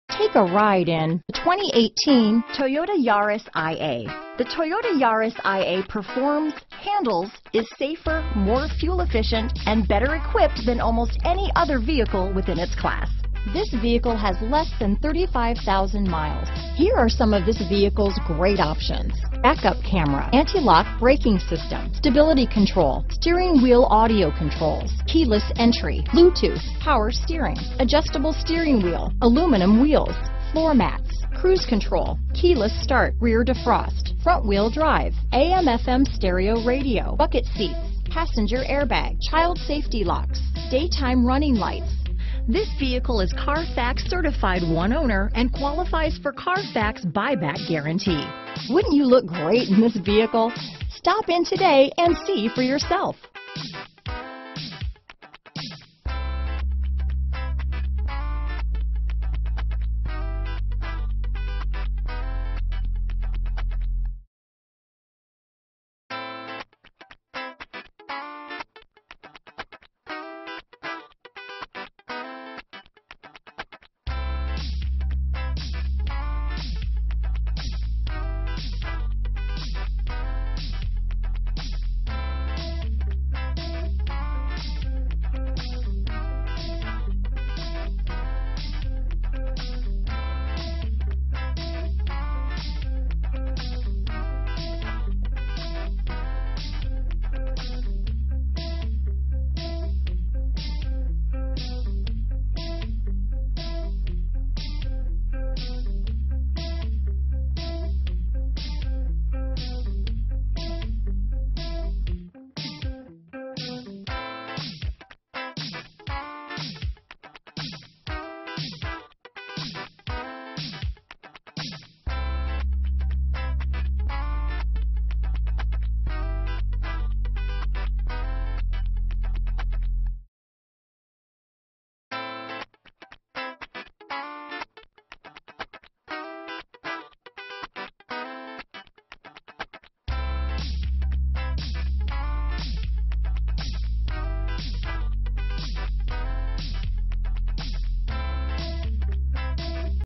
2018 Toyota Yaris iA Longwood, Orlando, Lake Mary, Sanford, Daytona Beach, FL LJ062323A
Black Used 2018 Toyota Yaris iA available in Longwood, Florida at Parks Toyota. Servicing the Orlando, Lake Mary, Sanford, Daytona Beach, FL area.
2018 Toyota Yaris iA Base - Stock#: LJ062323A - VIN#: 3MYDLBYV8JY300365

Parks Toyota
1701 South Woodland Boulevard

Sophisticated, smart, and stylish, this 2018 Toyota Yaris iA turns even the most discerning heads. With a Regular Unleaded I-4 1.5 L/91 engine powering this 6-Speed transmission, it is an understated assertion of your dominion over the open road. It's outfitted with the following options: Wheels: 16 x 5.5 Alloy, Wheels w/Machined w/Painted Accents Accents, Variable intermittent wipers, Urethane Gear Shift Knob, Trunk Rear Cargo Access, Trip computer, Torsion Beam Rear Suspension w/Coil Springs, Tires: 185/60R16 AS, Strut Front Suspension w/Coil Springs, and Steel Spare Wheel . Visit Deland Toyota at 1701 S Woodland Blvd, Deland, FL 32720 today.